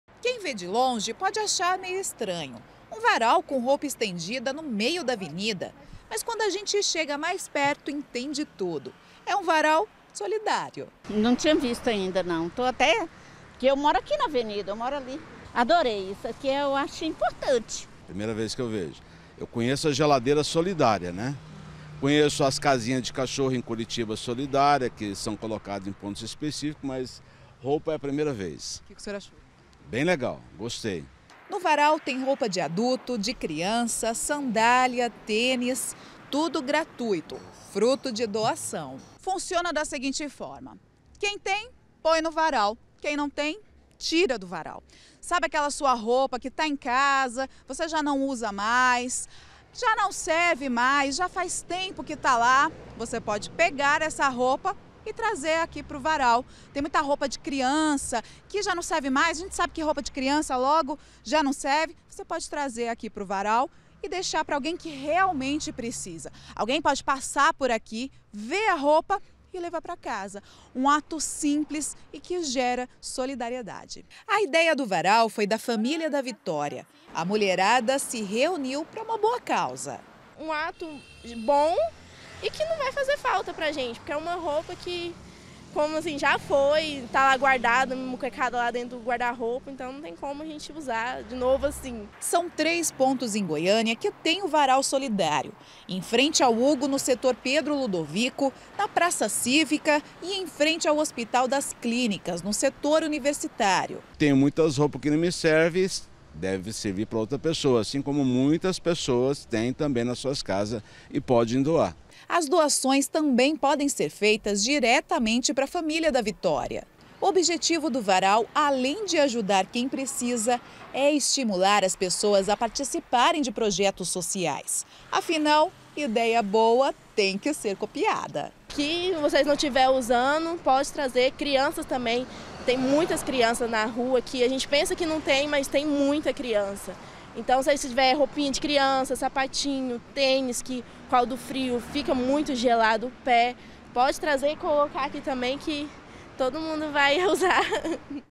PROJETO SOLIDÁRIO LEVA ROUPAS USADAS PARA AS RUAS
Um grupo de amigas levou para as ruas um projeto solidário. Varais foram estendidos em locais públicos com roupas usadas. quem não tem o que vestir, tem ali a oportunidade de se proteger do frio.